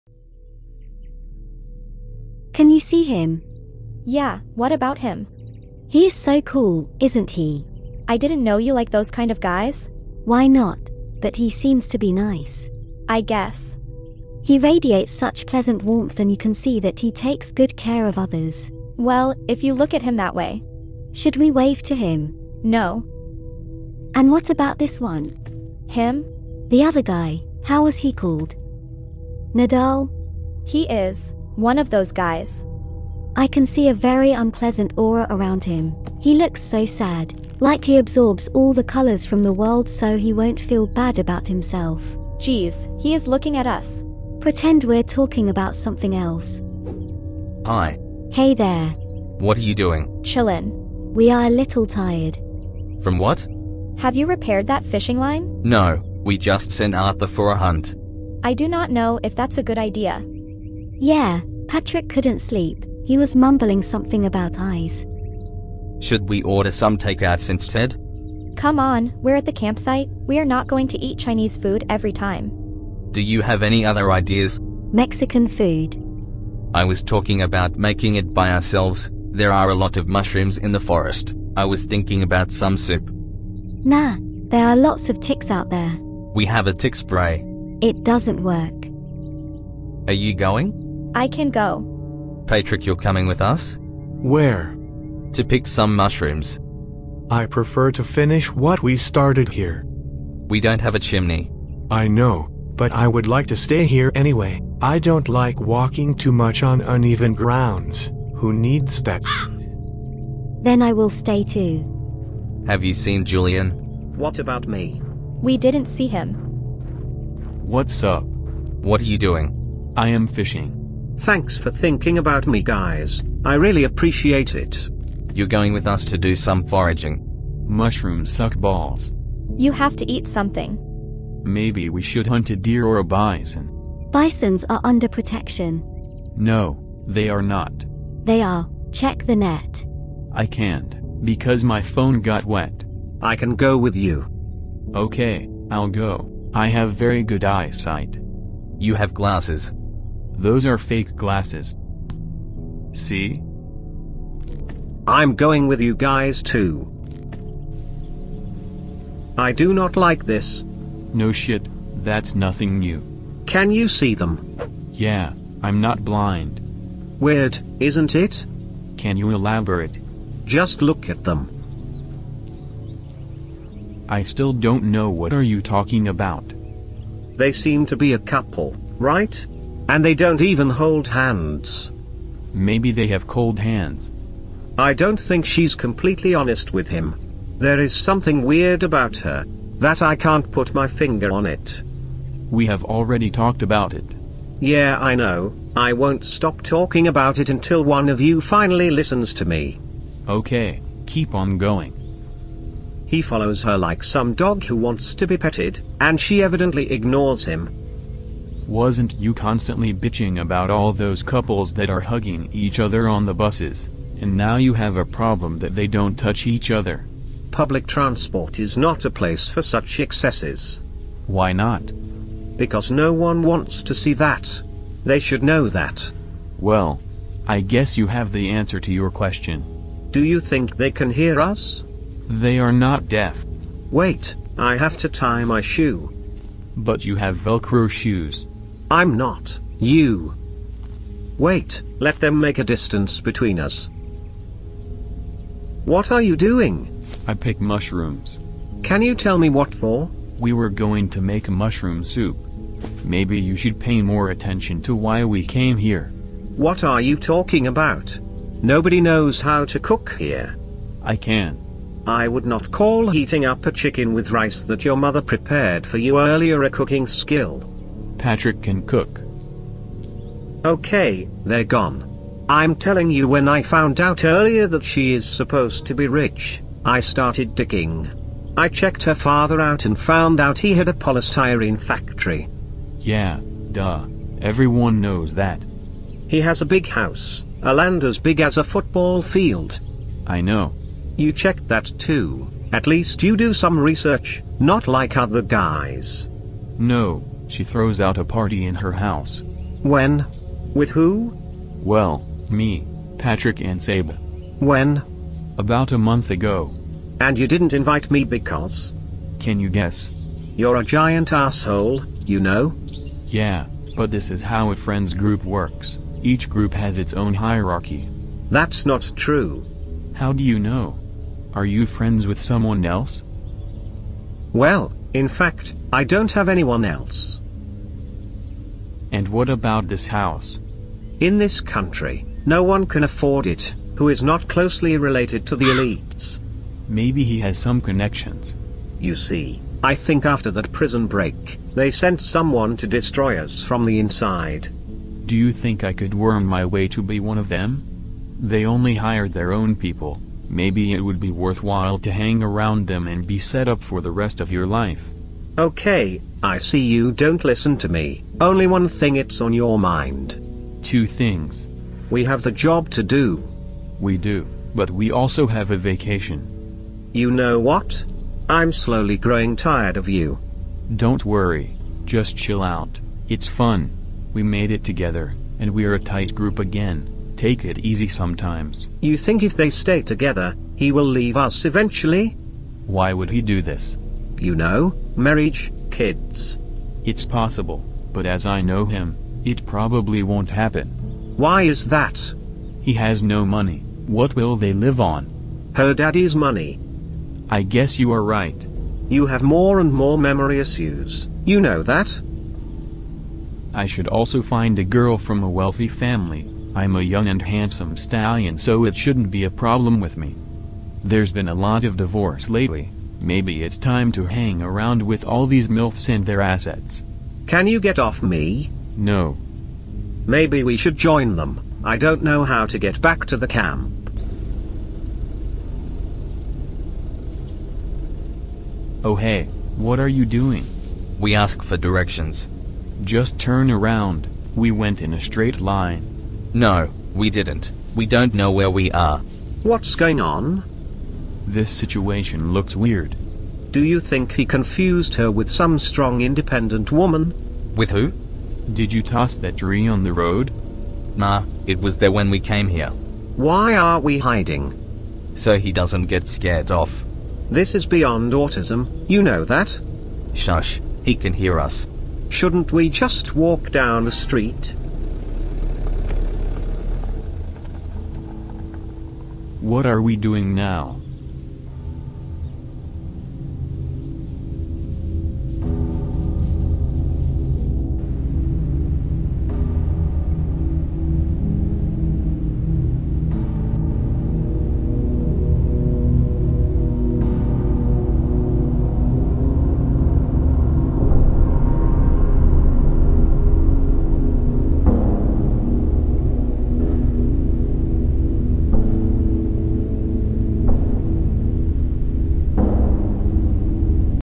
That One Friend That Have A Problem With Your Girlfriend
There is no end to the nagging. Constant repetition of talking points, pointing out the smallest mistakes of others. Turning others against you. That's the guy that called yourself your best friend. But is he right? Who knows...